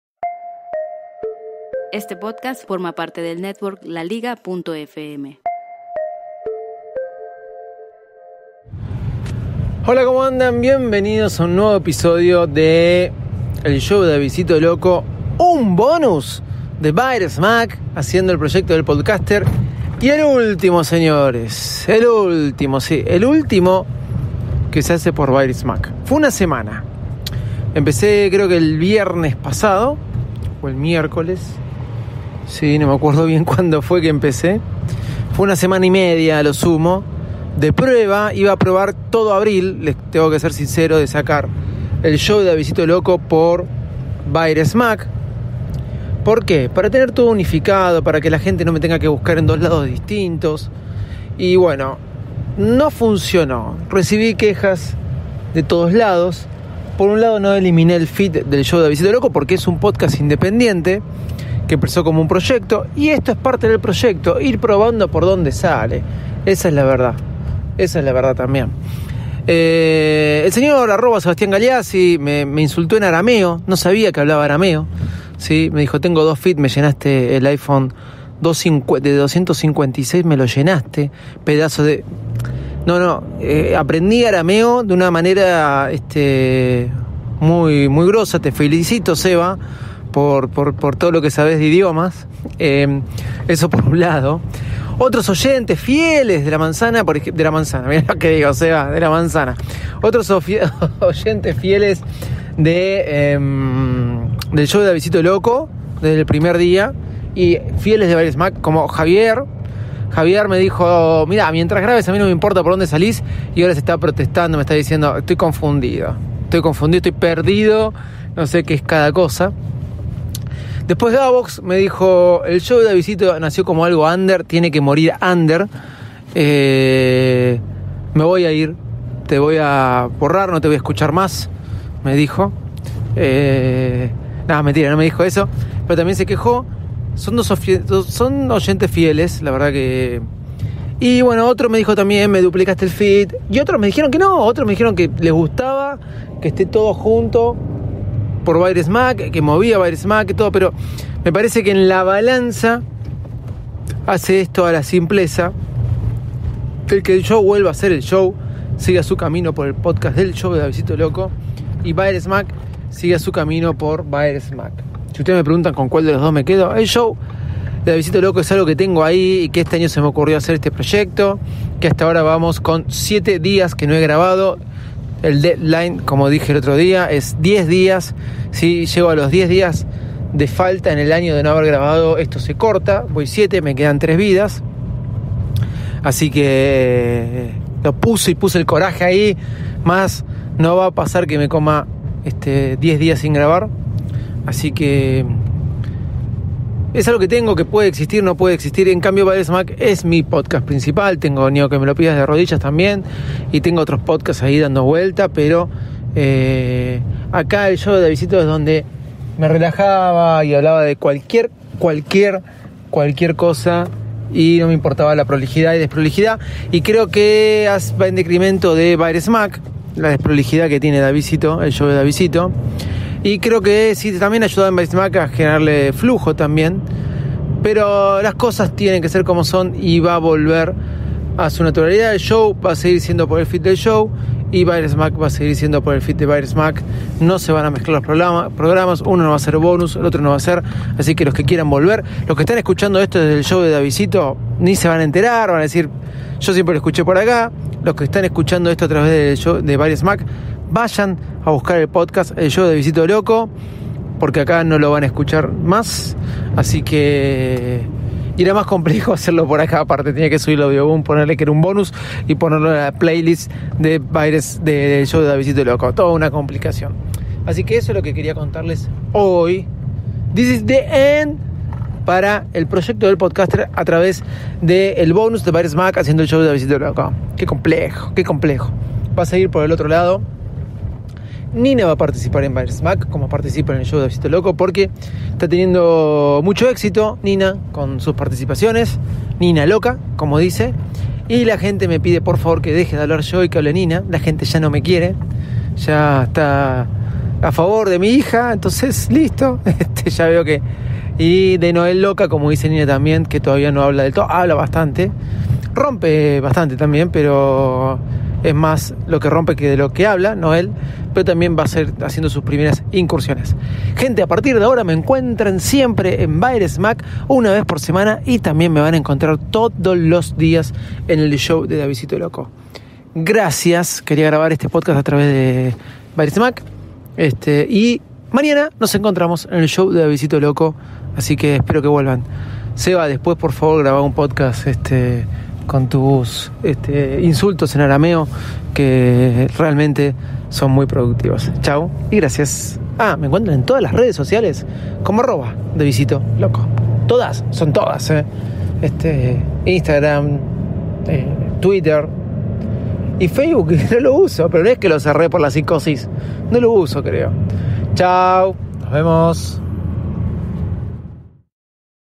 Día 93 ElProyectoDelPodcaster “el último”
Este es un BONUS de BairesMac llamado “El Show De @Davicitoloco” presentando ElProyectoDelPodcaster, grabar todos los días del 2018 un episodio.

En épocas donde todo es efímero, Snapchat, stories de Instagram,

Youtubers, nosotros que somos podcaster grabamos todos los días “algo”.

Acompáñanos en esta aventura de “El Show De @Davicitoloco”